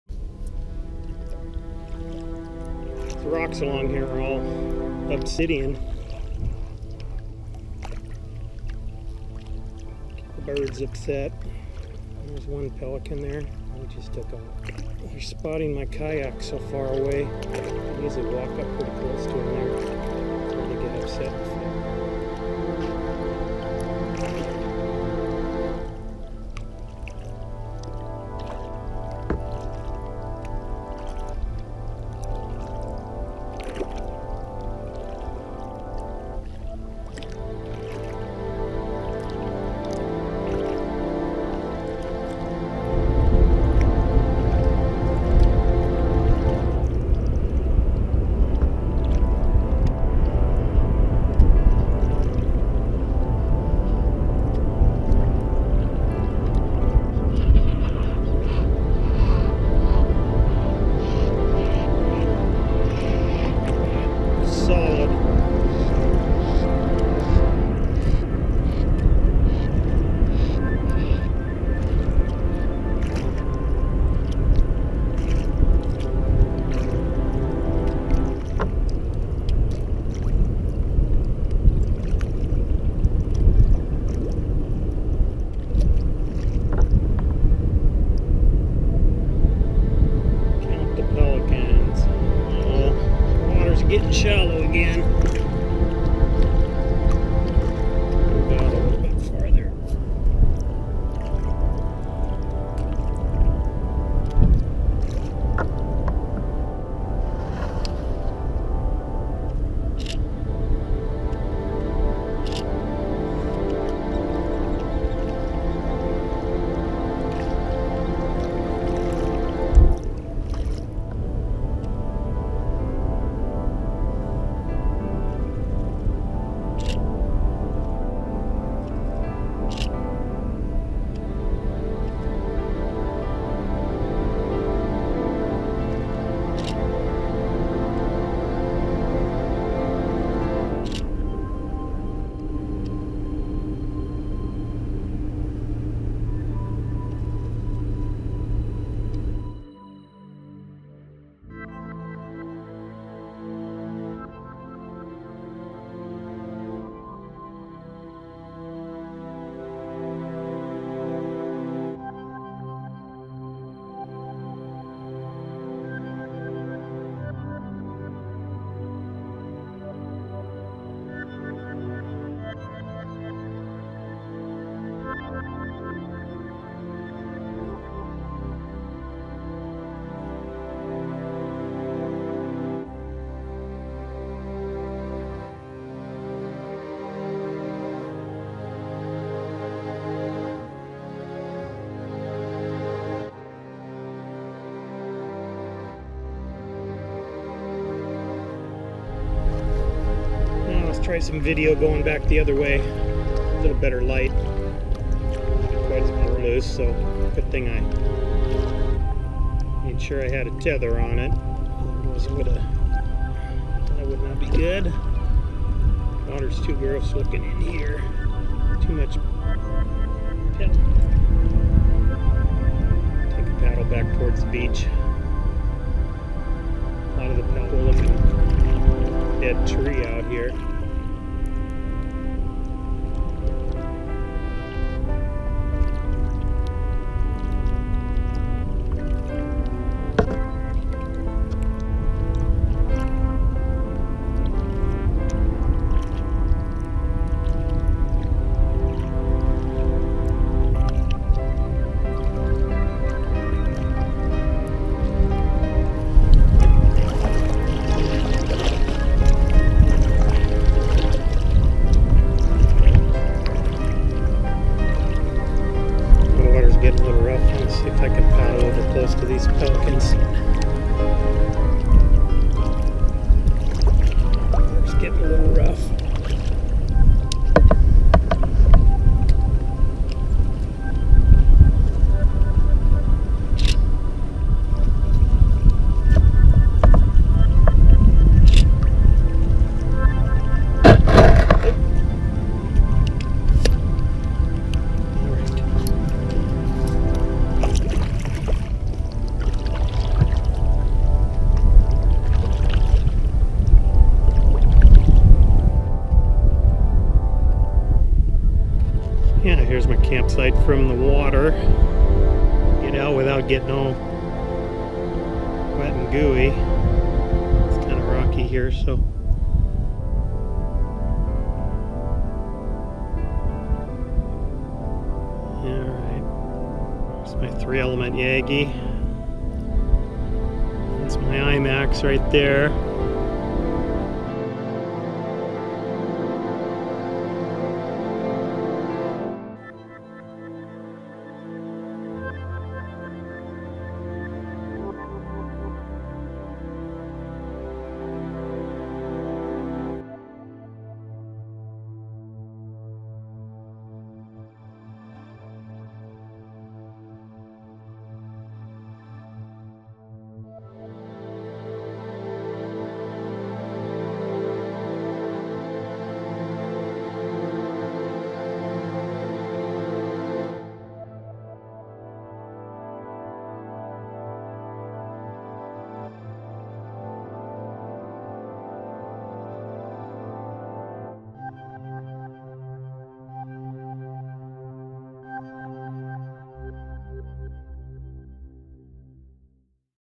Kayak Paddle, Salton Sea, Calif. Thousands of Pelicans Feb 2015
Out on my kayak checking out the bird life on the next bay to the west.  Found a pelican convention going on with thousands of brown pelicans lining the beach.  By far the most I have seen all together.  I had dozens of birds flying over me, so how I avoided getting poop bombed is a miracle.